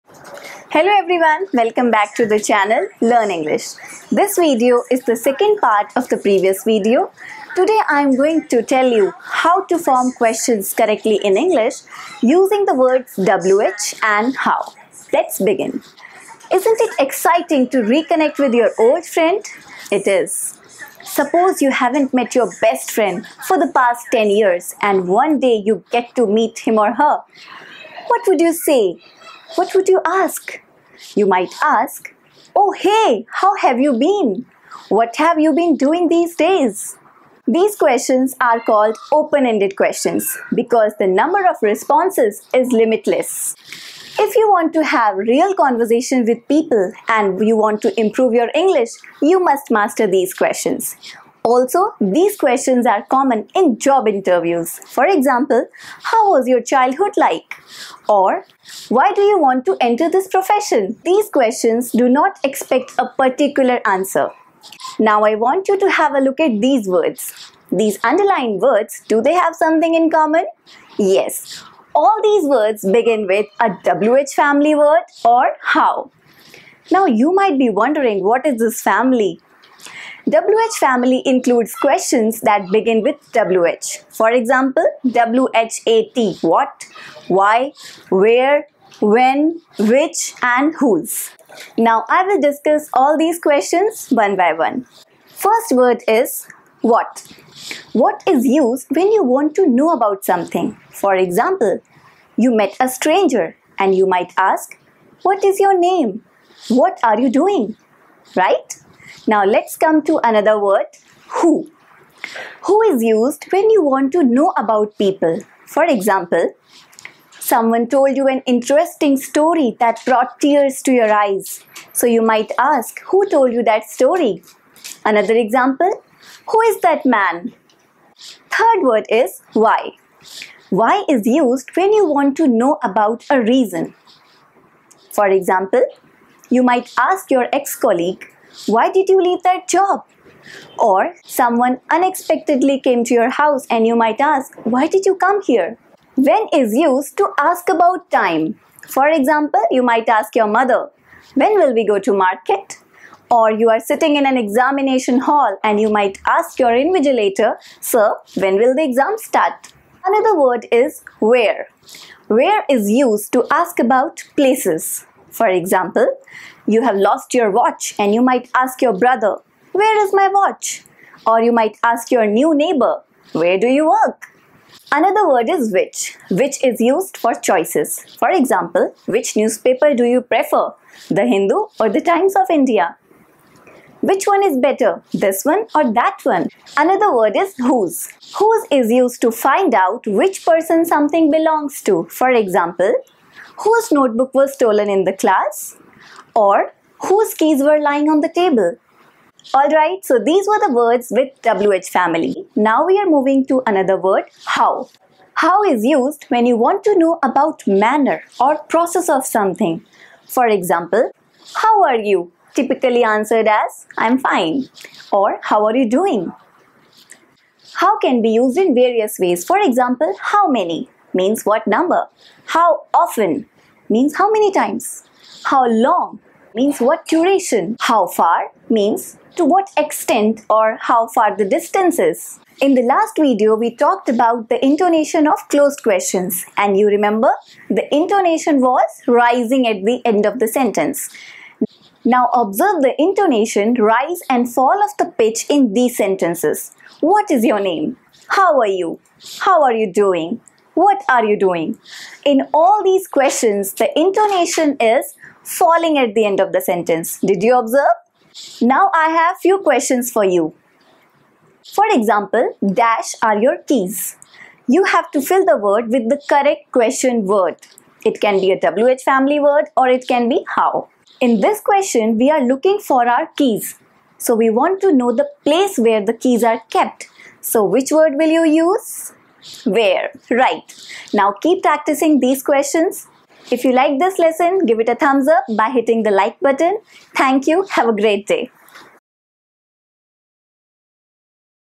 Want to form questions correctly ? - WH words and How
Do you want to improve your English ? Then, you are at the right place. In this lesson, I have explained how WH words and How can be used to form questions. These are open ended questions. To know about the other type of questions, you can watch the previous video

Learn the proper way to ask a question and understand the difference between open ended and closed questions. After watching this video, you'll be able to use the question words correctly and with proper intonation.
Learn new vocabulary with our channel everyday. Learn English for free.